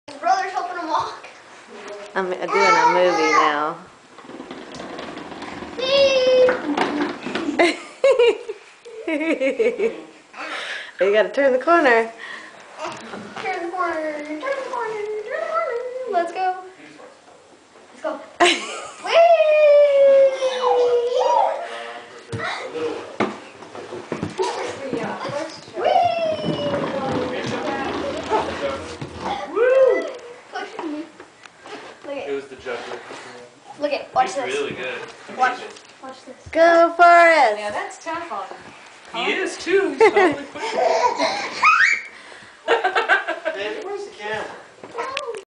fofo and nono
Forrest pushing noah